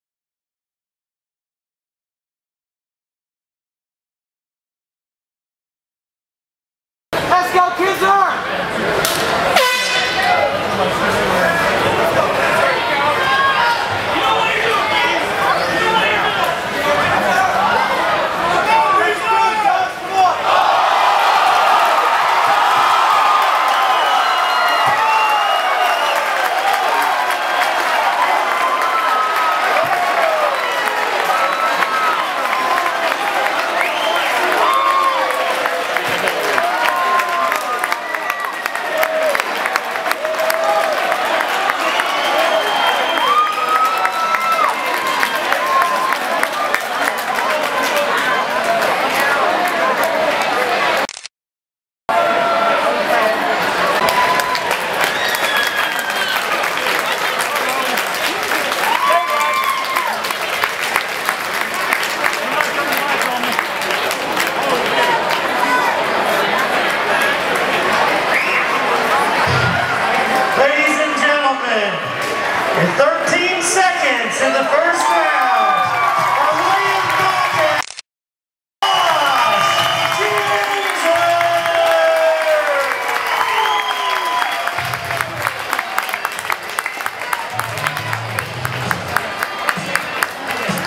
Josh Kinser KO.wmv
Josh Kinser's 13 seconds KO at Colosseum Combat XVI
Taken from my Canon 7D DSLR camera with a F2.8 300mm lens. This isn't from a HD video camera. I resized it down from 259 MB using windows movie maker.